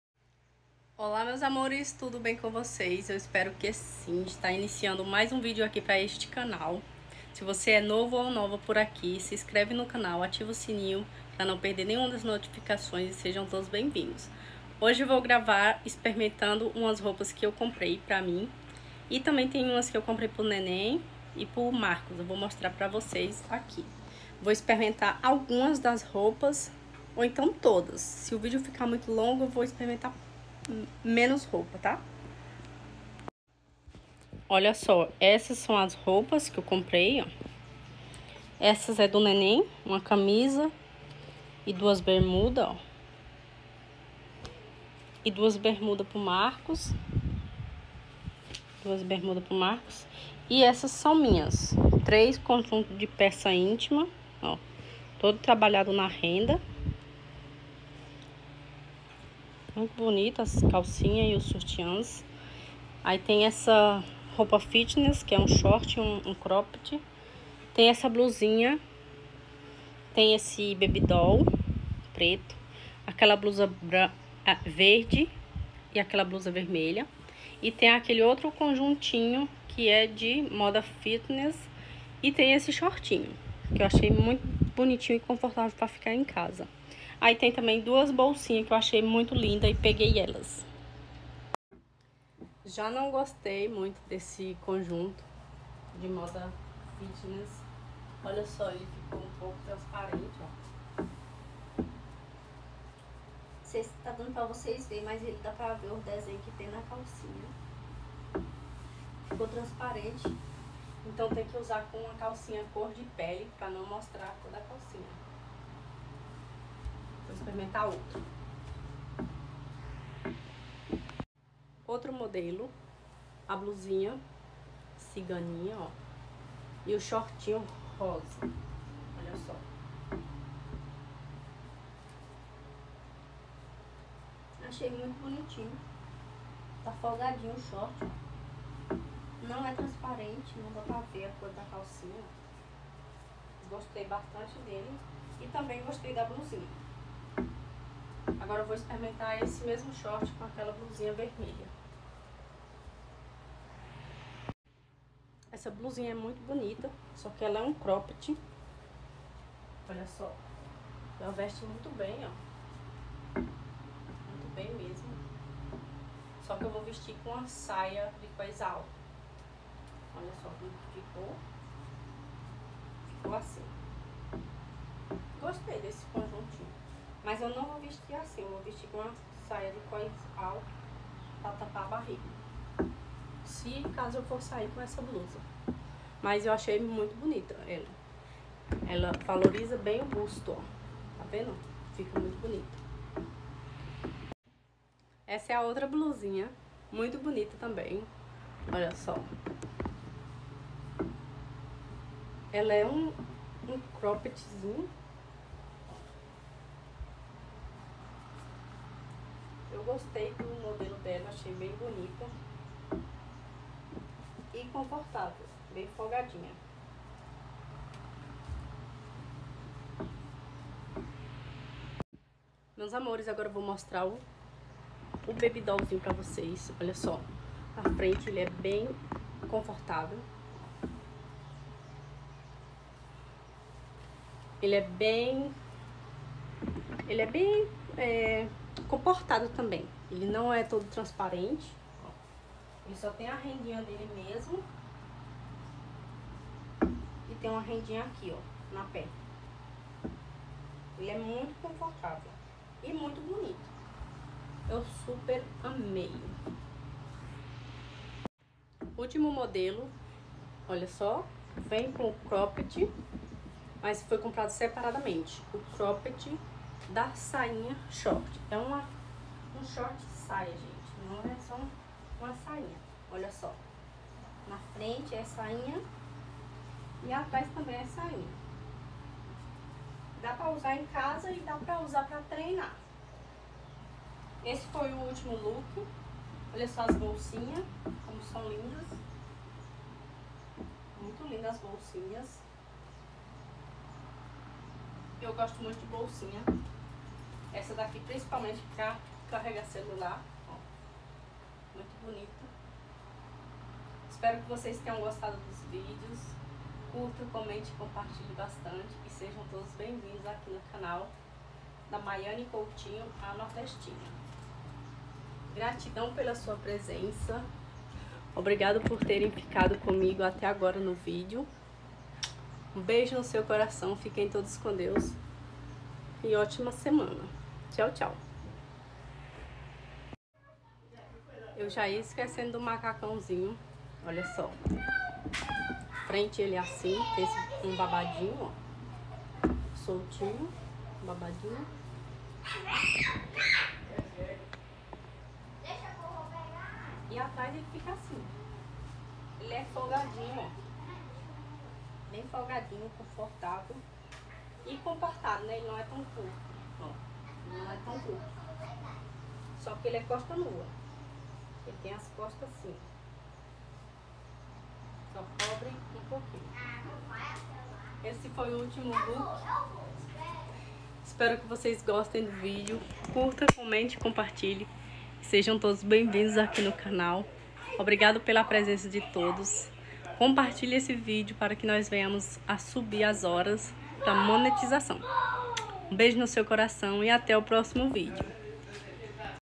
PROVANDO ROUPAS DE ACADEMIA E OUTRAS QUE COMPREI
Olá meus amores hoje trago pra você eu vestindo as roupas que comprei hoje tudo ficou 380$ esqueci de fala no vídeo 😂

Se inscreve
Sejam bem-vindos ❤️